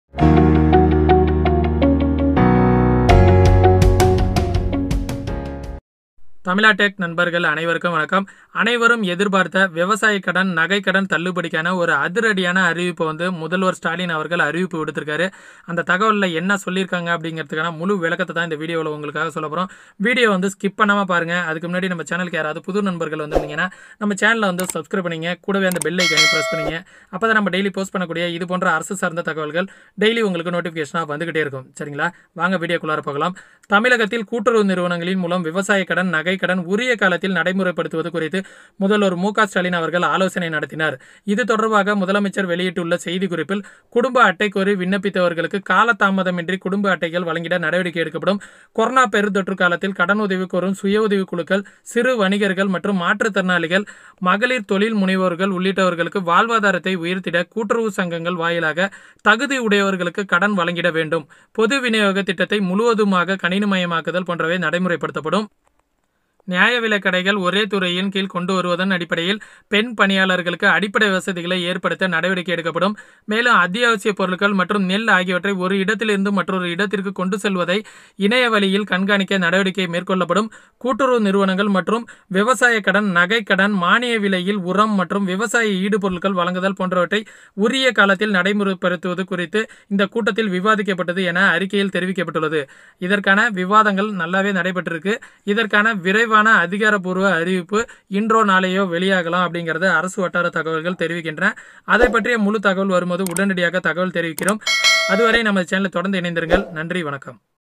Breaking news : விவசாயக் கடன், நகைக்கடன் தள்ளுபடி..! முதல்வர் ஸ்டாலின் அதிரடி அறிவிப்பு..!!!!!
நாளை முதல் தமிழ்நாட்டில் ஊரடங்கு உத்தரவு புதிய கட்டுப்பாடுகள் என்ன?

இரண்டு கடன் தள்ளுபடி கான அரசாணையை பதிவிறக்கம் செய்ய இங்கே கிளிக் செய்யவும் Click download